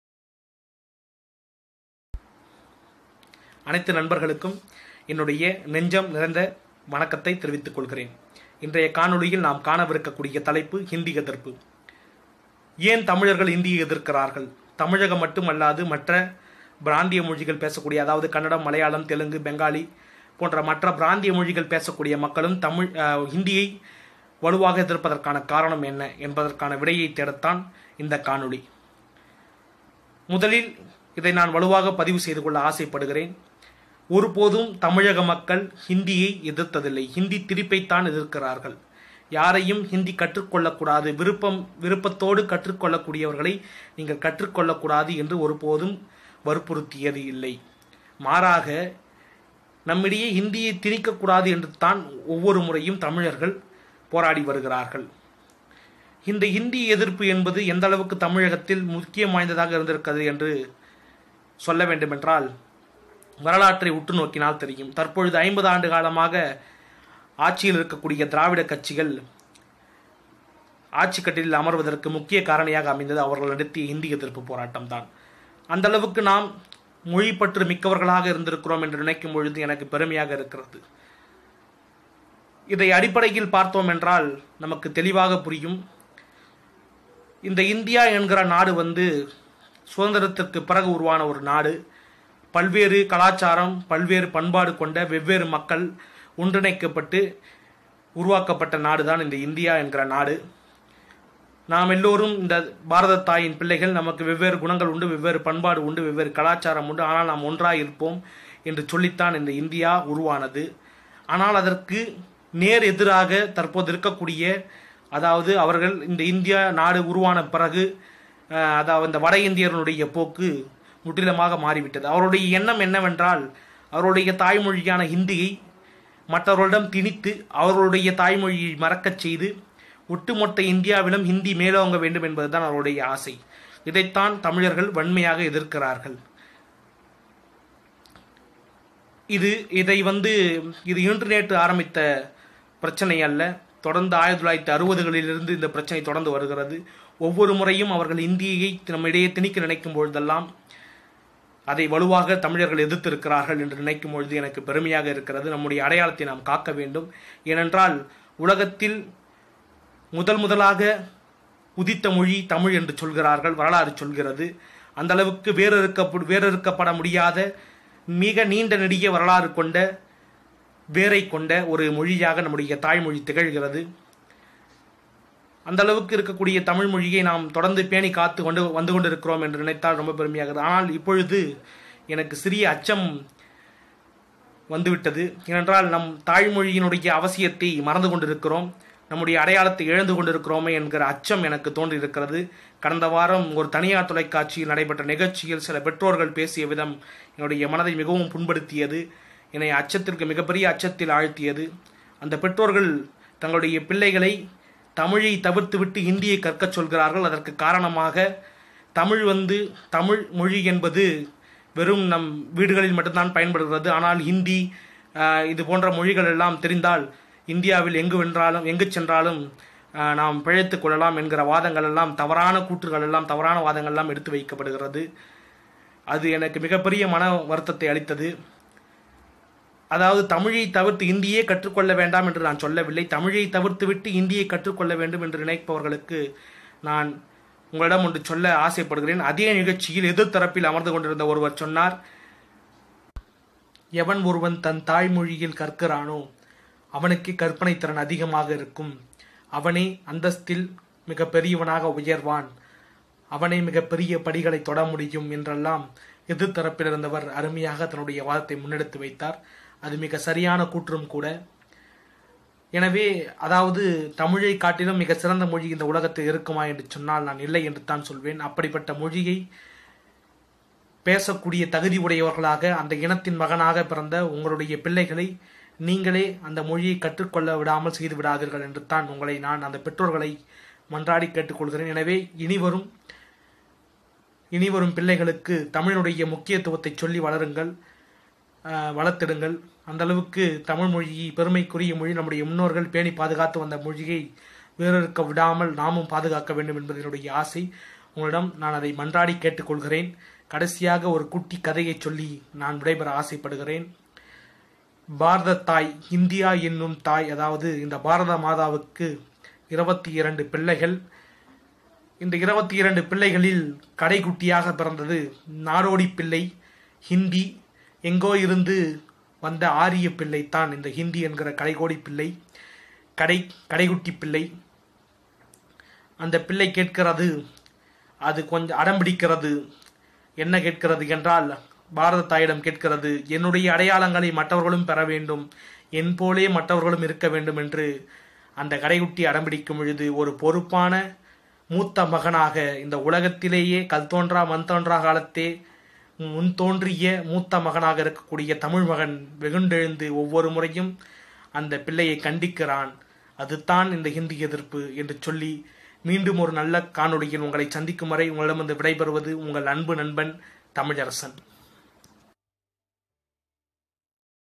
HINDI OPPOSITION | ஹிந்தி எதிர்ப்பு | SAVE IDENTITY | New Comer
இந்த காணொளியில் ஹிந்தி எதிர்ப்பு குறித்தும்,தாய் மொழியின் அவசியம் குறித்தும் விவரிக்கப் பட்டுள்ளது.